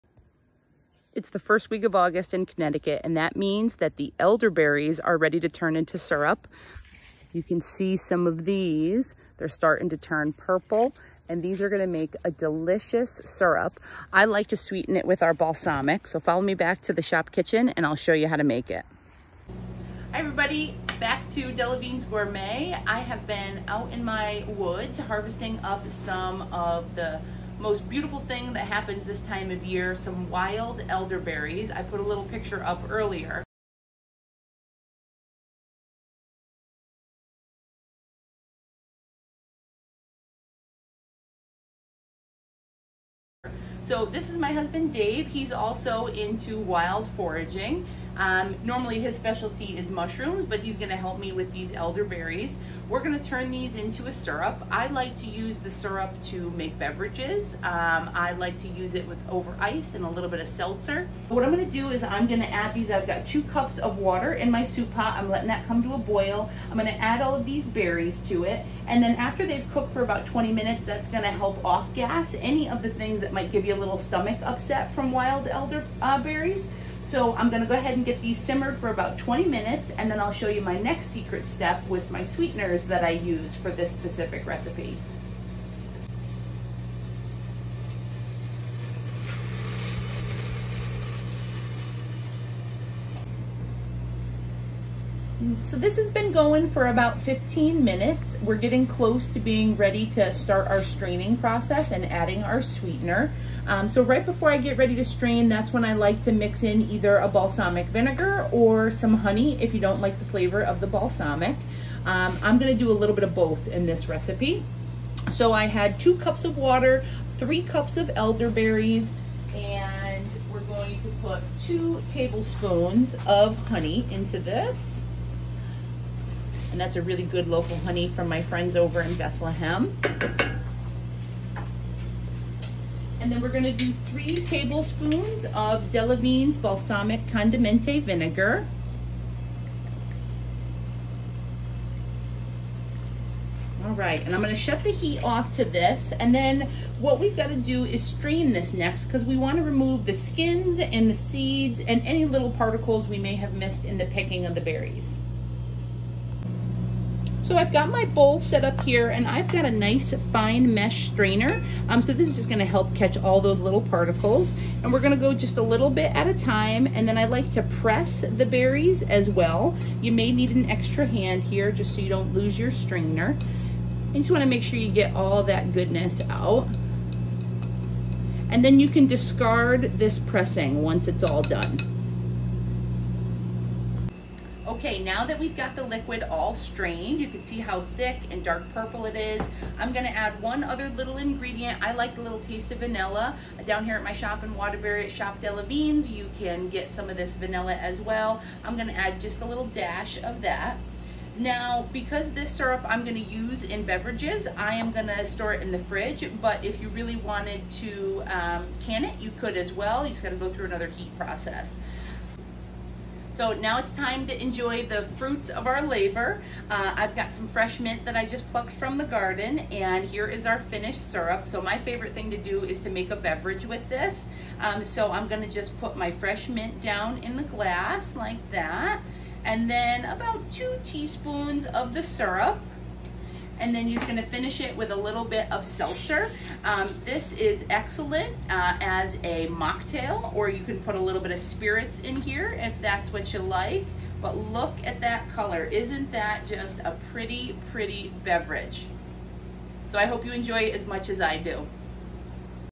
Wild Elderberry Harvest & Elderberry Balsamic Syrup Recipe
Local Connecticut Wild foraged Elderberries and how to prepare and enjoy elderberry syrup sweetened with Balsamic Vinegar and honey.